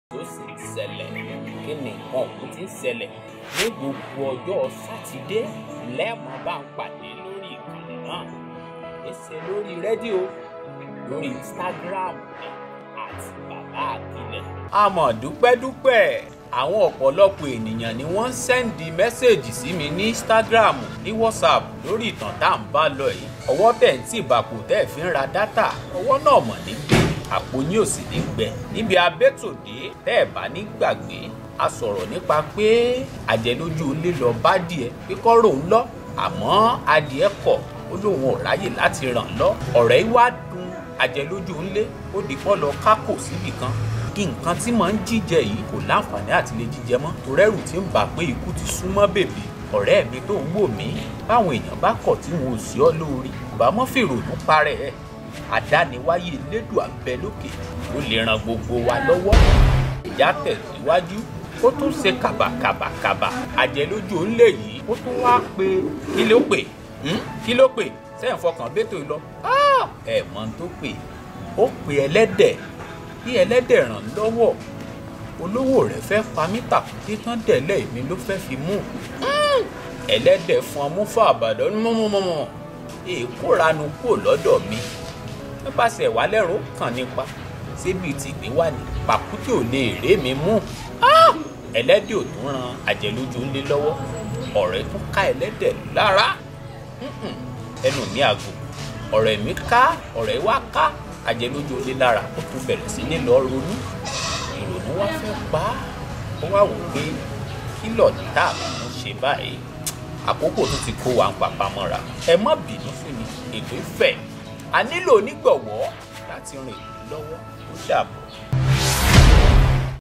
so si n sele Season 1 Ep 3
Discover the untold depths of what goes on in life that remains unnoticed.  So si n sele? Is a series that tell stories of what still goes on in life without much notice. It's full of tragic, lessons and fun. Watch this interesting series and kindly follow @babaakile for the next episodes of so si n sele!?

*Só Sì ń Sęlè!?*
Another interesting series from *Baba Akile StreetTv📺* *Só Sì ń Sęlè* is an instagram series you won't dare to miss.